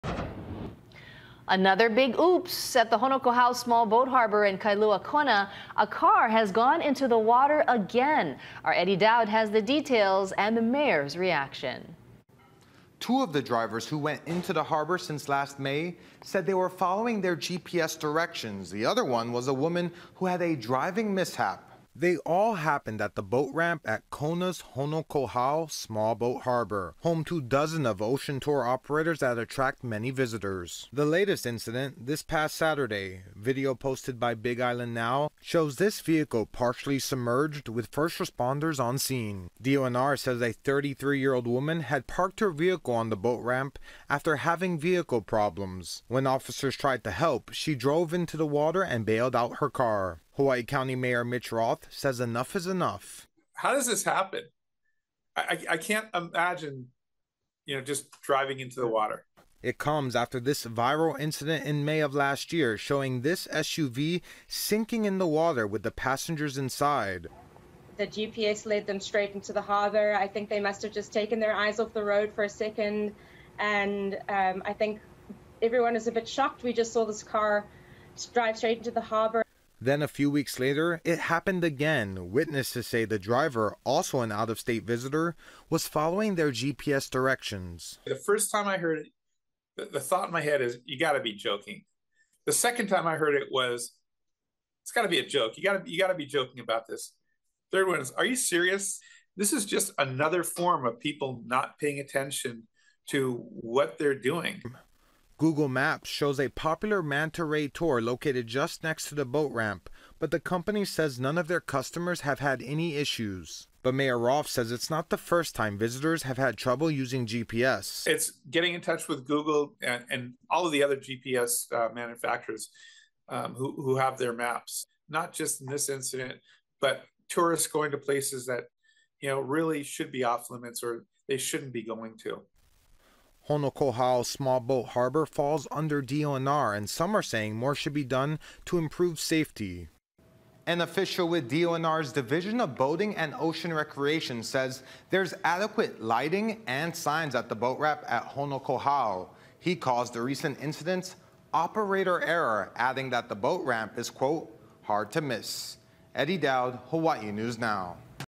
Third vehicle goes into a Kailua-Kona harbor in 13 month span
In just over one year, three vehicles have driven into the water at Honokohau Small Boat Harbor in Kailua Kona.